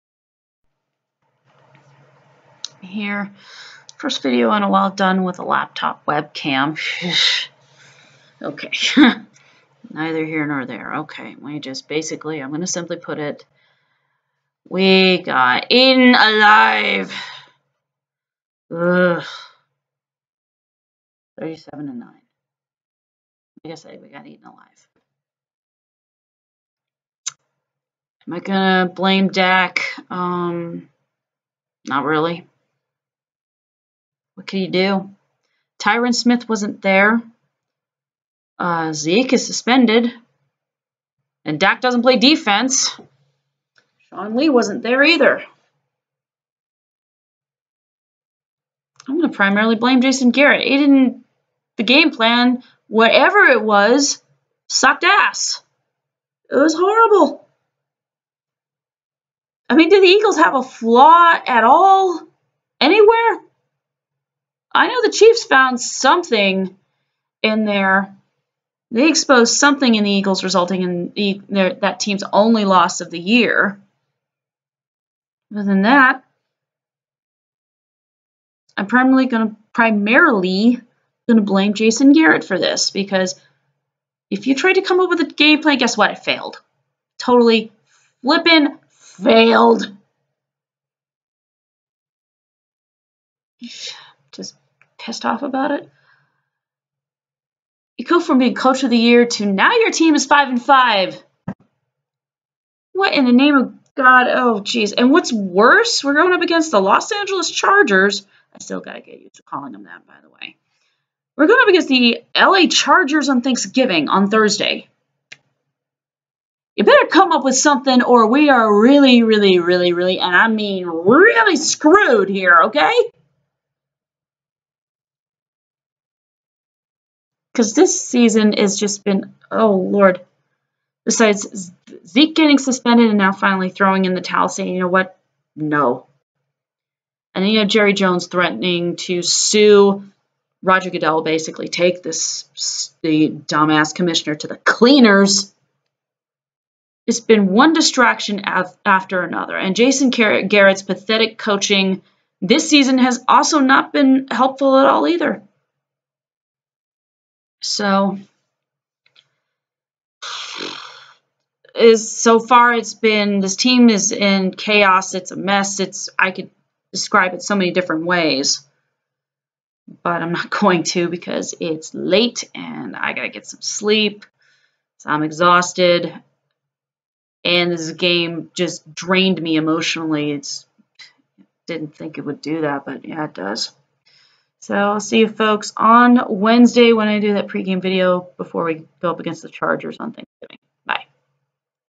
cowboys/eagles (postgame) (week 11)
I talk about the embarrassing loss to the eagles & who I blame for the loss.